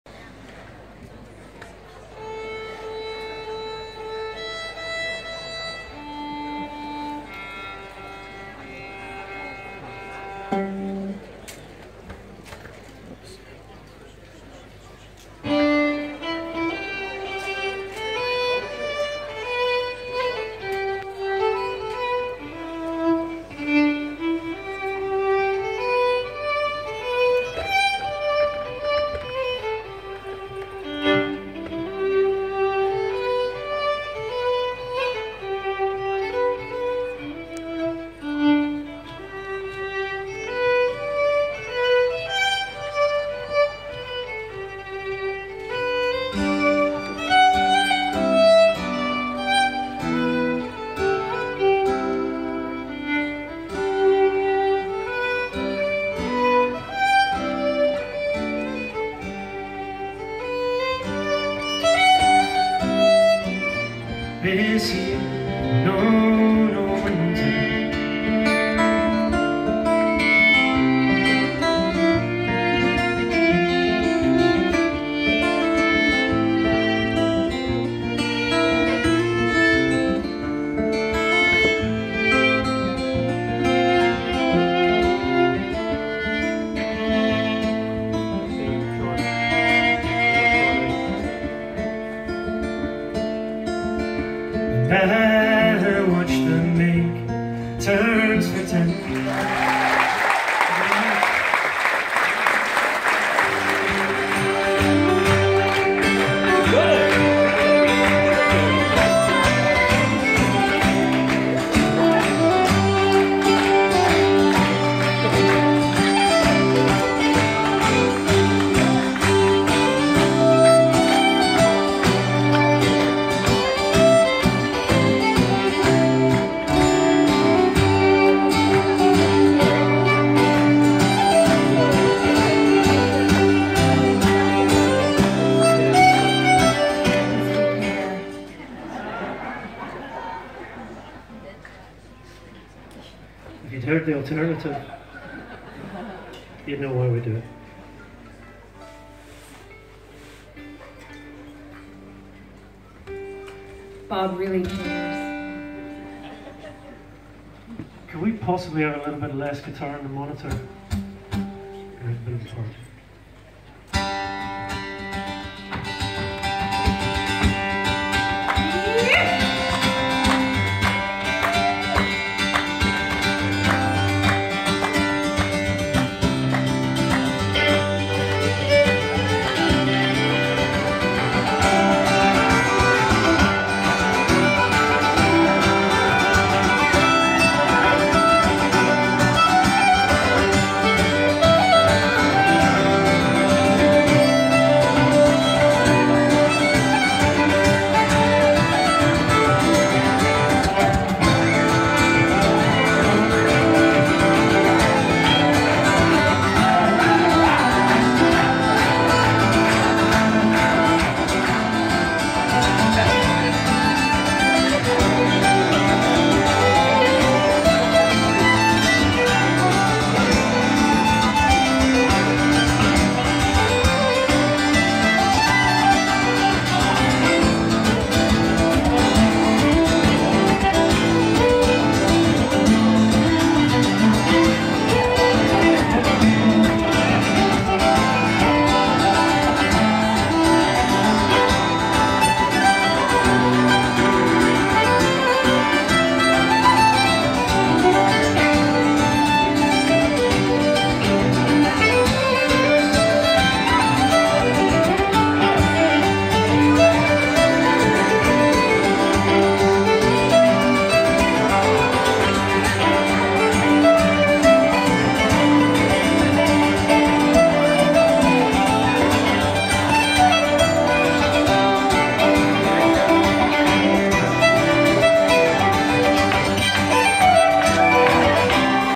Allen Heath QU32 with twin iPad Pros
Allen Heath Qu32 live with twin iPad Pros using Mac Airport Router.
Video taken from Samsung Galaxy cell phone.